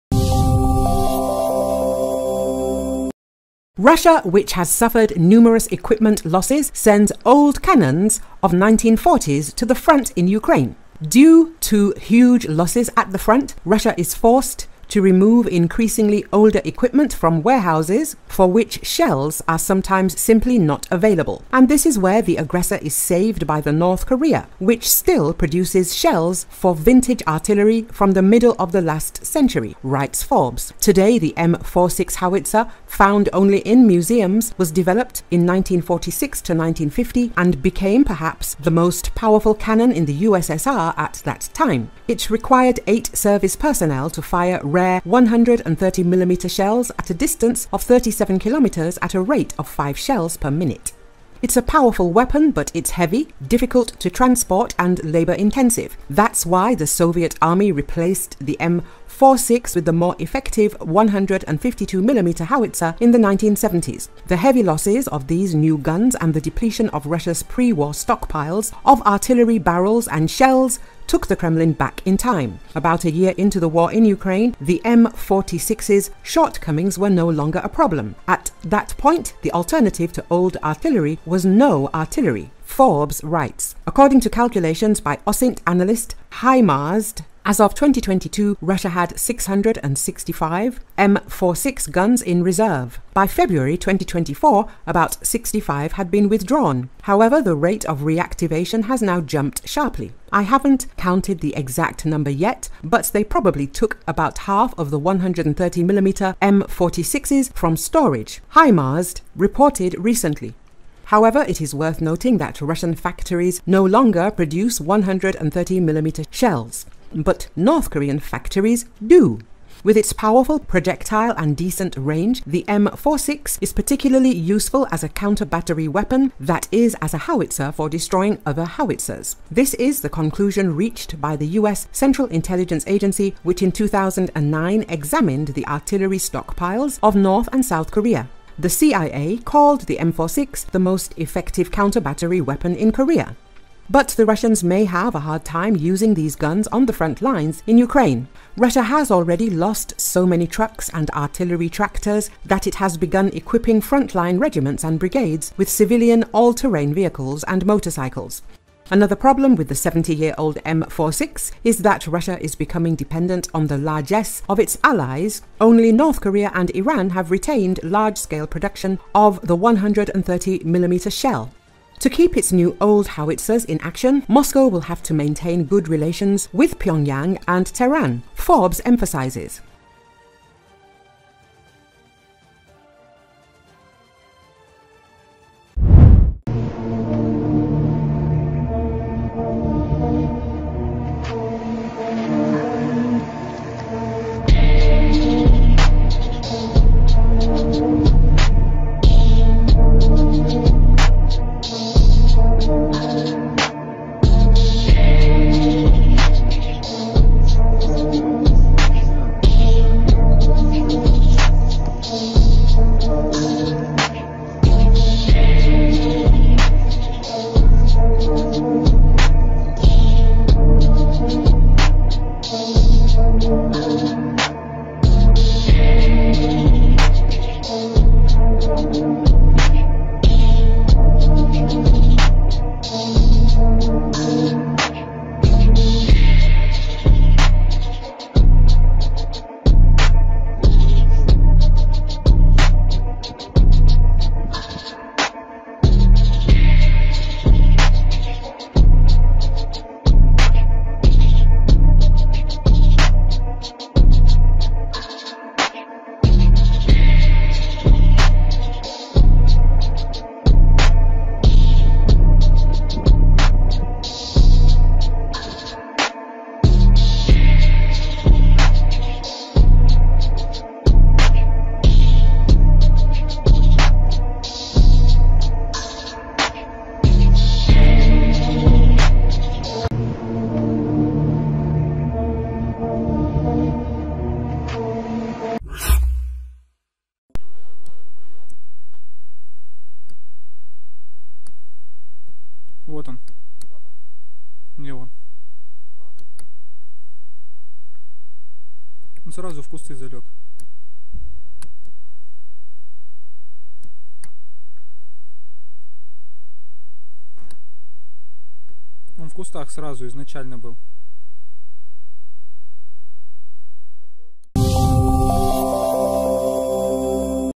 Russia, which has suffered numerous equipment losses, sends old cannons of 1940s to front in Ukraine
Due to huge losses at the front, Russia is forced to remove increasingly older equipment from warehouses, for which shells are sometimes simply not available. And this is where the aggressor is saved by the North Korea, which still produces shells for "vintage" artillery from the middle of the last century, writes Forbes.

Today, the M-46 howitzer, found only in museums, was developed in 1946-50 and became perhaps the most powerful cannon in the USSR at that time. It required eight service personnel to fire rare 130-mm shells at a distance of 37 km at a rate of five shells per minute.

"It's a powerful weapon, but it's heavy, difficult to transport, and labor-intensive. That's why the Soviet army replaced the M-46 with the more effective 152mm howitzer in the 1970s. The heavy losses of these new guns and the depletion of Russia's pre-war stockpiles of artillery barrels and shells took the Kremlin back in time. About a year into the war in Ukraine, the M-46's shortcomings were no longer a problem. At that point, the alternative to old artillery was no artillery," Forbes writes.